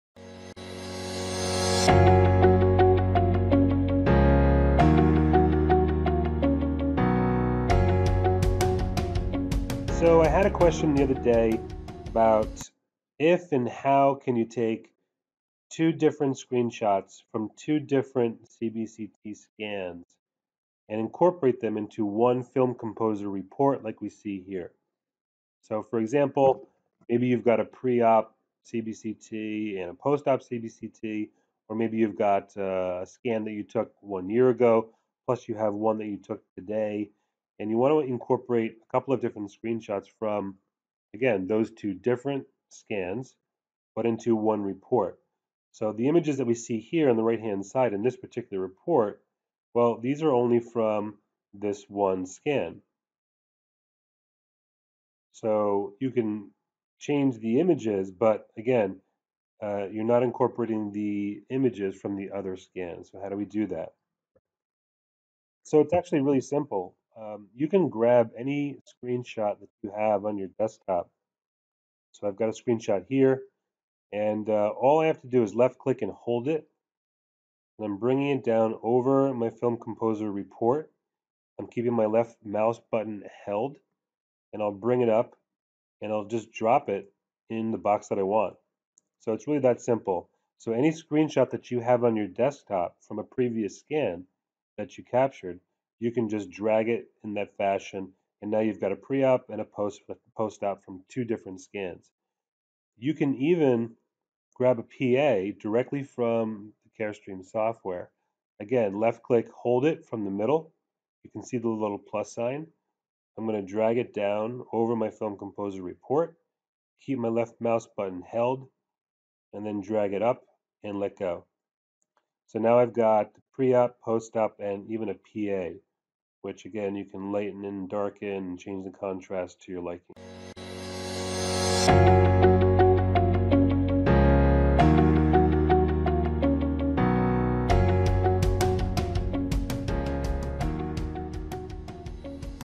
Carestream 3D Imaging - Film Composer Report
This video will show CS 3D Imaging users how to incorporate multiple screen shots from different scans and even PAs into one Film Composer Report.